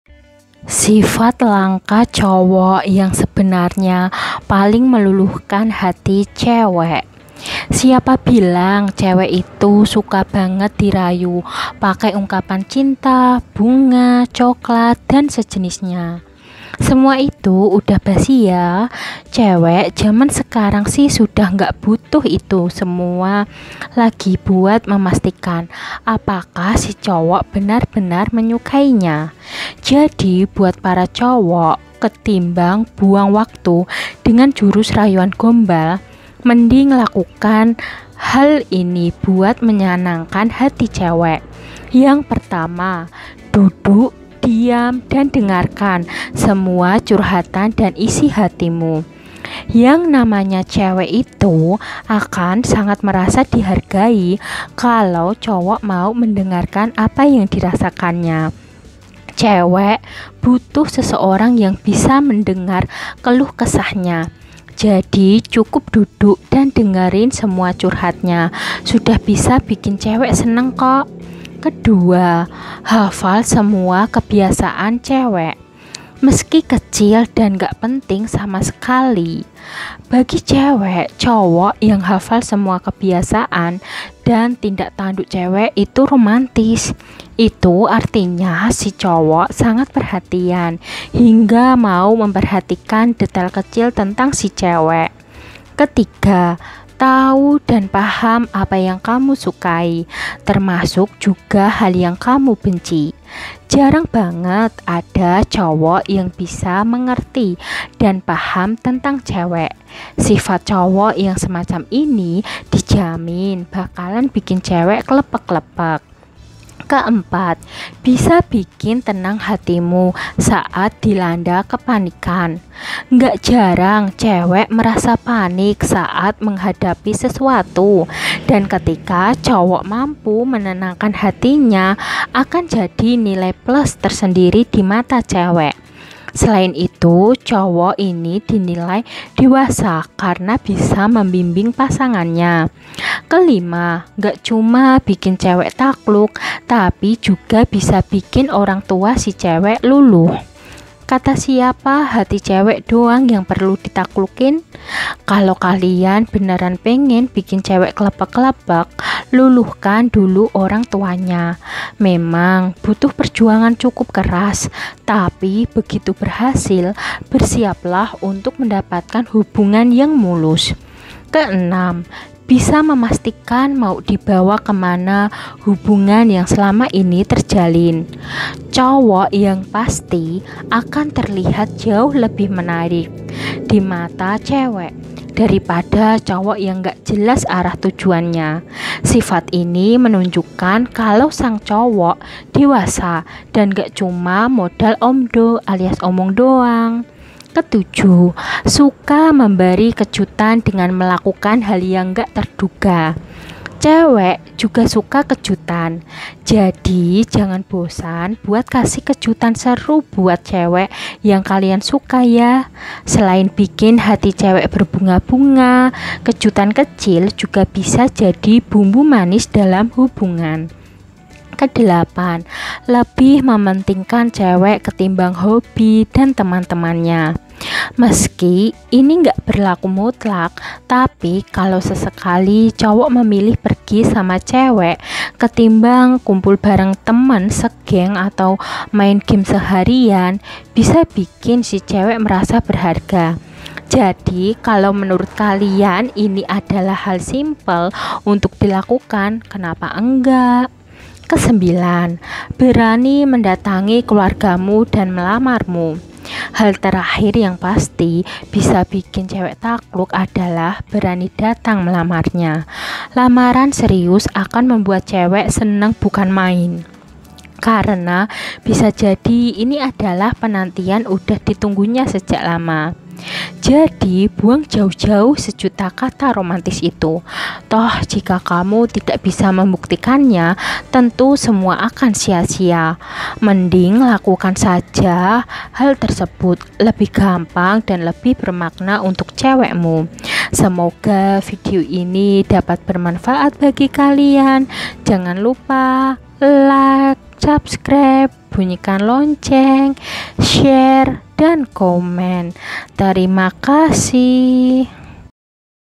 RAHASIA COWOK COOL | Sifat Langka Cowok yang Paling Meluluhkan Hati Cewek
Siapa bilang cewek itu suka banget dirayu pakai ungkapan cinta, bunga, coklat dan sejenisnya? Semua itu udah basi. Cewek zaman sekarang sih sudah gak butuh itu semua lagi buat memastikan apakah si cowok benar-benar menyukainya. Jadi buat para cowok, ketimbang buang waktu dengan jurus rayuan gombal, mending lakukan sembilan hal ini buat menyenangkan hati cewek.